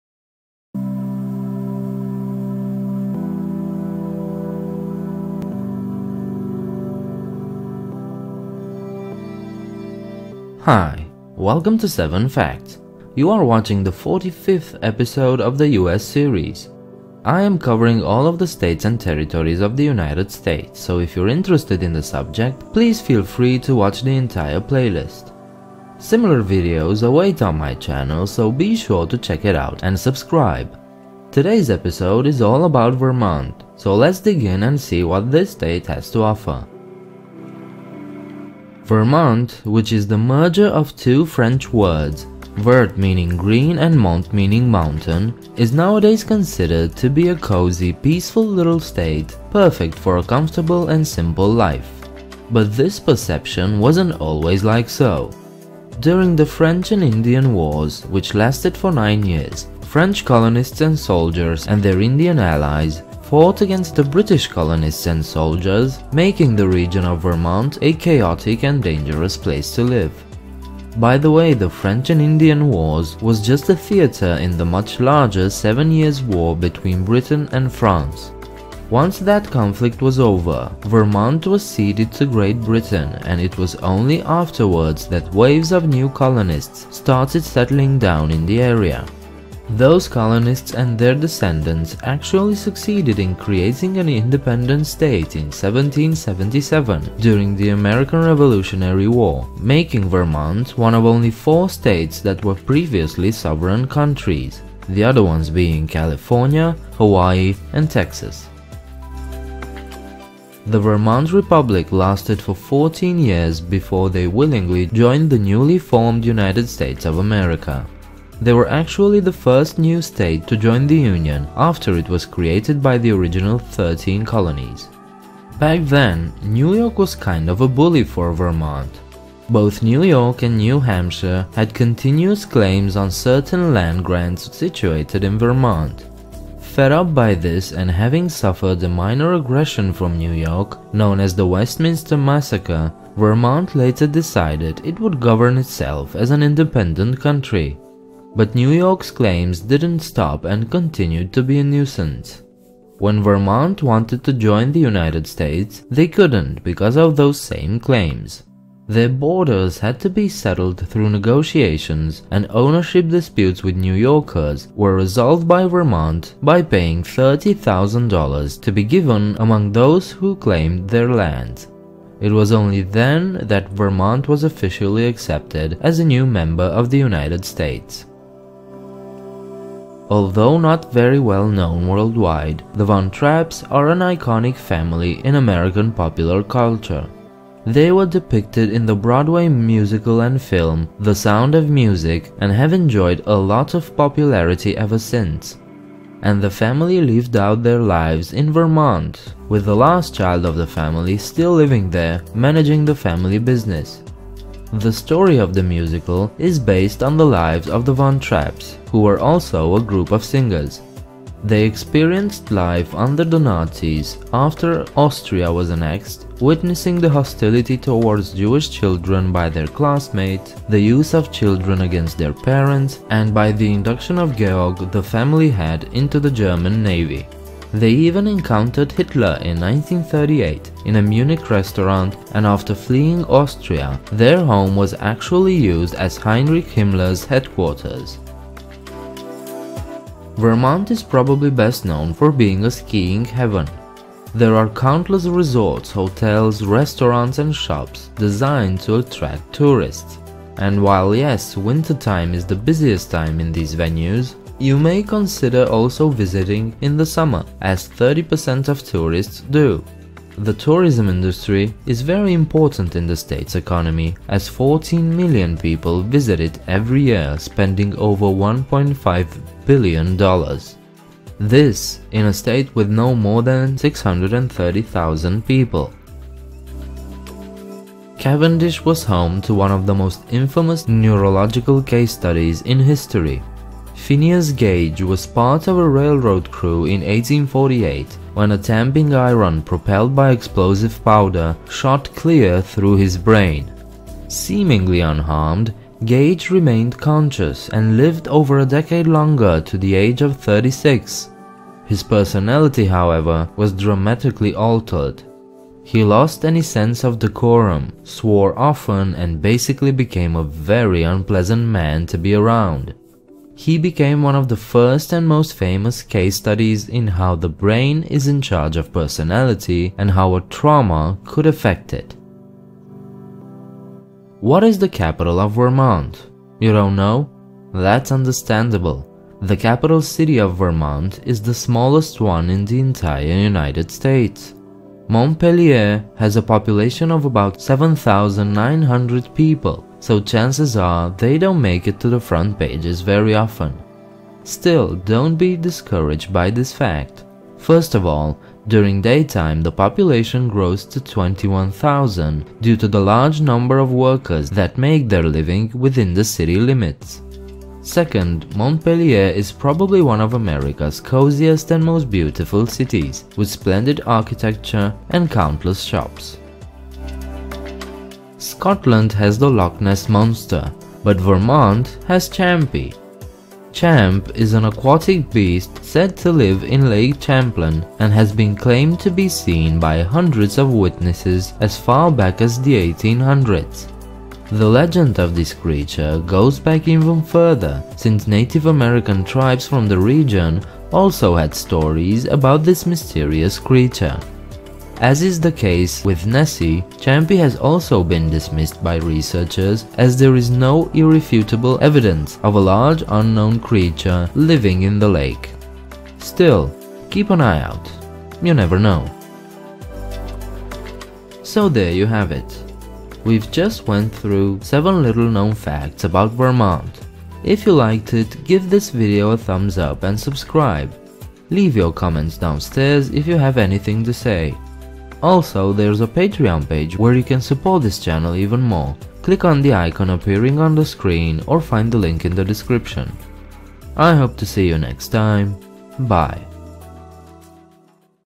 7 Facts about Vermont
In this video you can find seven little known facts about Vermont.

More information about the video content bellow:
1. Officially recognized as the 14th state in 1791, Vermont (taken from the French word “vert,” or green, and “mont,” for mountain) has long represented bucolic, comfortable living.  It wasn’t always so cozy. The French and Indian War made the territory which would later be known as Vermont a chaotic place of unrest; it wasn't until the arrival of peacetime in 1763 that waves of settlers decided to give it a shot. While they were motivated to populate the area, resources like saw mills were limited; early log houses had bark roofs and flooring.

2. They had a beef with New York. The neighboring state had tried to claim the land as its own in the 1770s, even escalating the tension to include a small-scale invasion and threats of seizure by brandishing land titles in 1775. The resulting battle, dubbed the Westminster Massacre, left two dead.  Bitter over New York’s aggression, Vermont opted to call itself a republic in 1777, electing its own president (Thomas Chittenden), making its own money, and writing a constitution that predated the U.S. version by ten years. By 1790, however, it relented and paid New York $30,000 in silver to get off its back.

3. The von Trapp family depicted in The Sound of Music are proud Vermont residents. Arriving in 1942, the family settled in Stowe and purchased a ski lodge: Johannes von Trapp, the eldest child, still operates the property.

4. While skiing is responsible for much of the state's tourism, over 30 percent of visitors come during the summer months to check out the parks and campsites, and to visit area lakes.

5. Cavendish was home to one of the most infamous neurological case studies in history. Phineas Gage was part of a railroad crew in 1848 when a tamping iron propelled by explosive powder shot clear through his brain. Seemingly unharmed, Gage remained conscious, and lived over a decade longer to the age of 36. His personality, however, was dramatically altered: he lost any sense of decorum, swore often, and became a case study in how brain trauma could affect personality.

6. Vermont’s state capital of Montpelier happens to be the smallest in the nation. With just over 8000 residents, it’s dwarfed by Albany’s 98,000.

7. It gave the U.S. its own version of the Loch Ness Monster. “Champ,” an aquatic beast said to live in Lake Champlain, was once pursued by P.T. Barnum and has been seen by hundreds of witnesses as far back as the 1800s. In 1982, Vermont wryly declared the lake a safe haven for the creature.

Intro Creator:
Pushed to Insanity